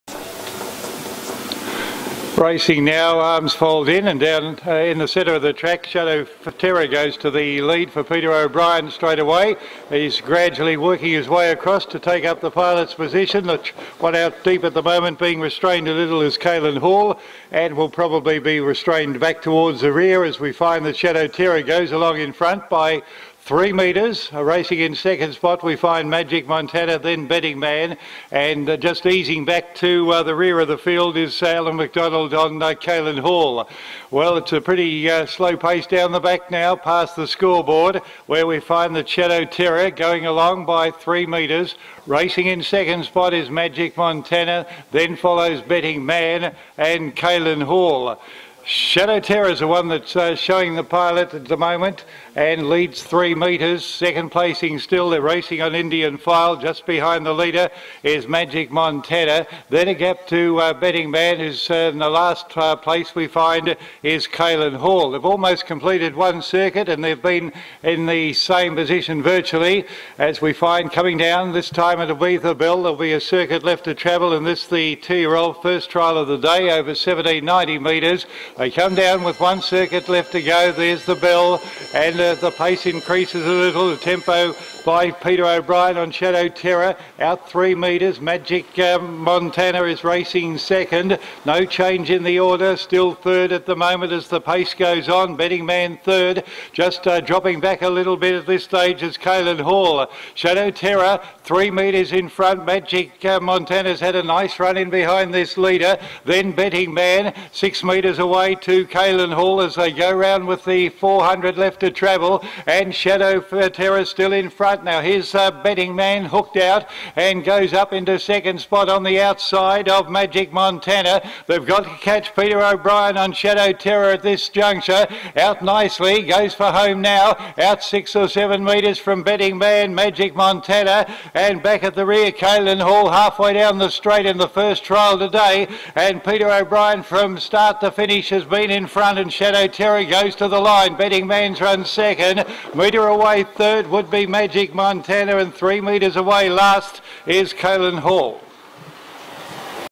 Mildura Harness Racing Club- 30 Jul 2017 Trial 1
Mildura Harness Racing Club-
30/7/2017 -- Trial 1
2YO -- 1790m -- Mobile

Time: 2.21
Margins: 4m x 1m
Mile Rate: 2.6.7
Last Half: 60.4